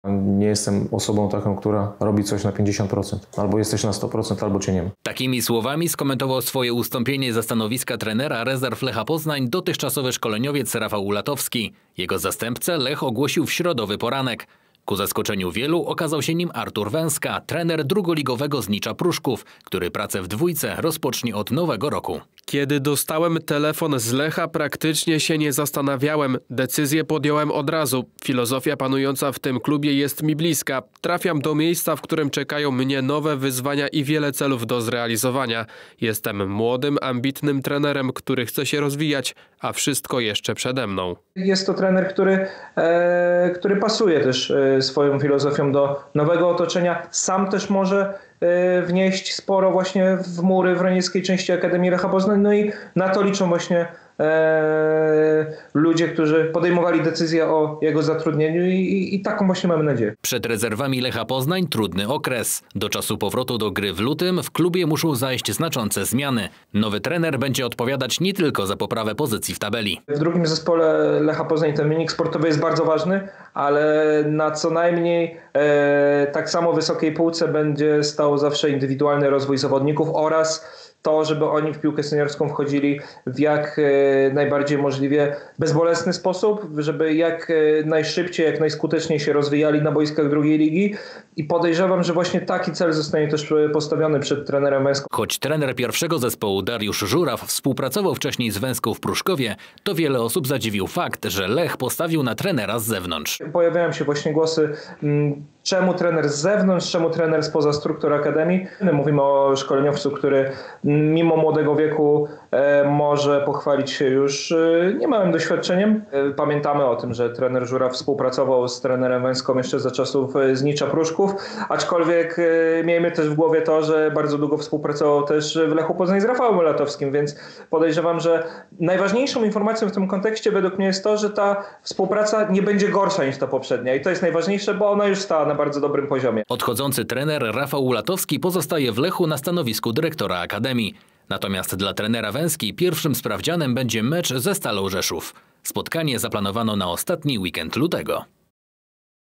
Rezerwy Lecha Poznań z nowym trenerem. Nie brakuje kontrowersji
Drugoligowe rezerwy Lecha Poznań będą miały nowego trenera. Wybór
następcy Rafała Ulatowskiego odbił się w środowisku szerokim echem. Autor materiału: Julian Antoniewicz